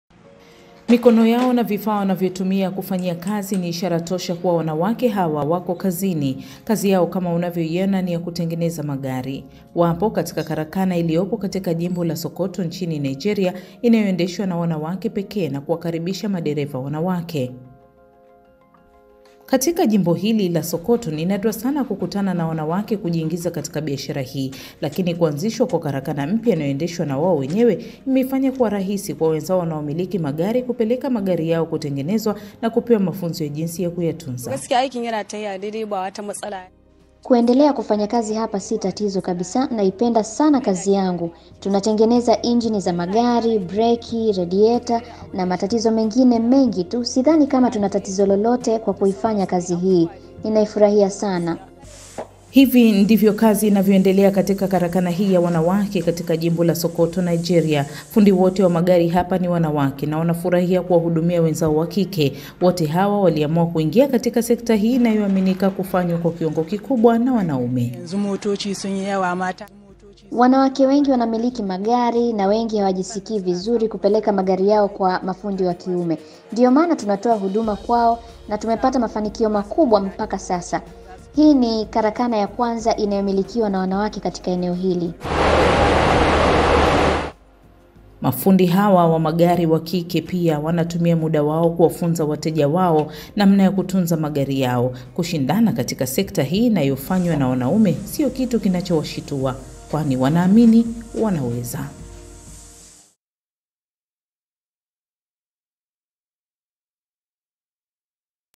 Karakana ya wanawake pekee yaanzishwa Sokoto-Nigeria
Wanawake wa Jimbo la Sokoto nchini Nigeria wamepata ujasiri wa kufungua karakana ya wanawake na kuamua kupambana na wanaume katika kazi ya utengenezaji magari.